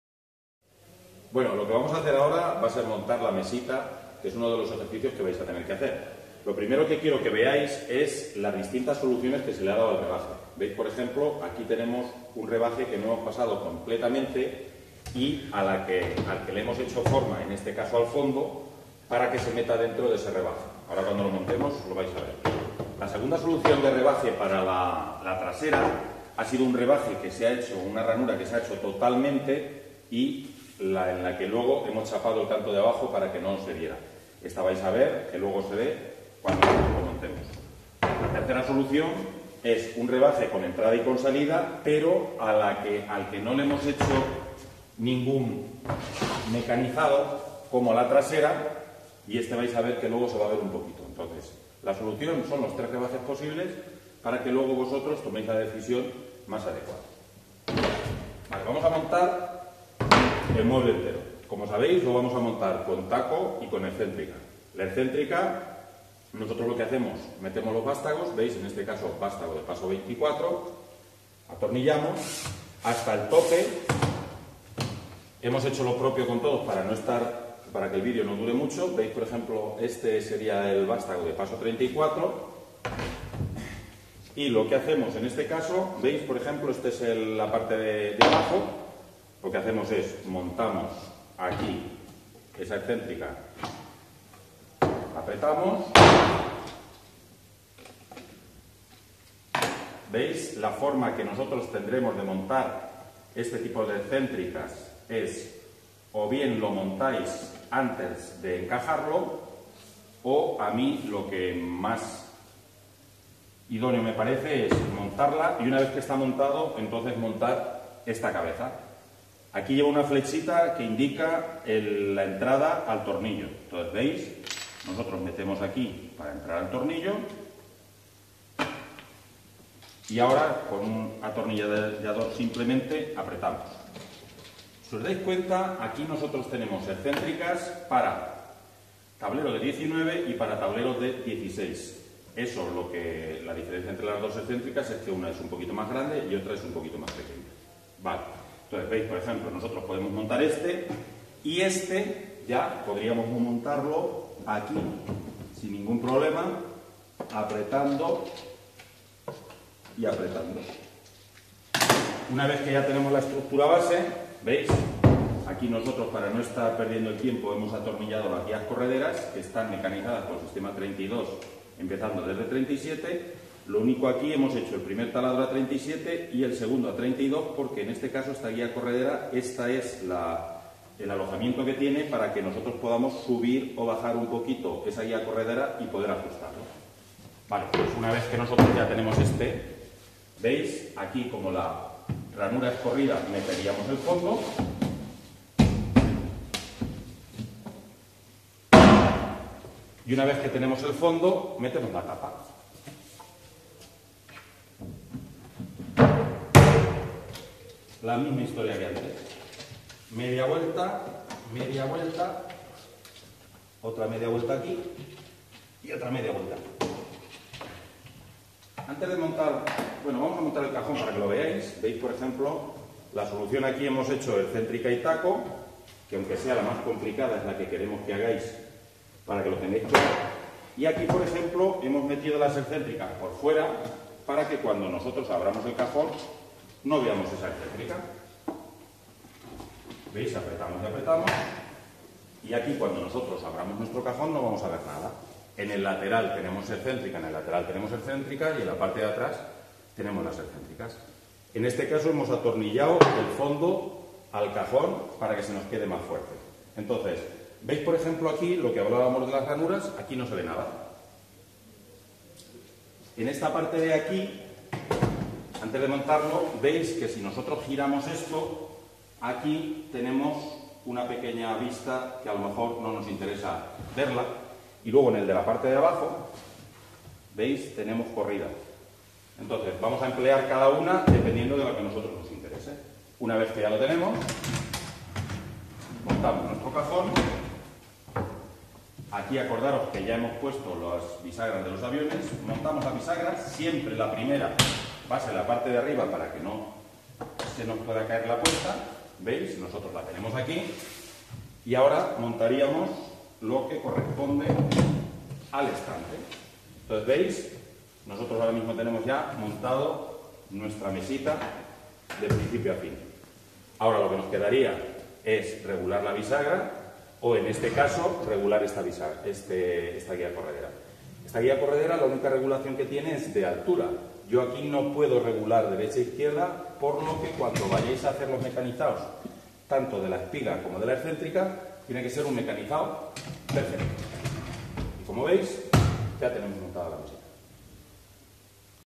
Mesita Excentrica Insti
Montaje de mesita con todos los mecanizados de taladro posibles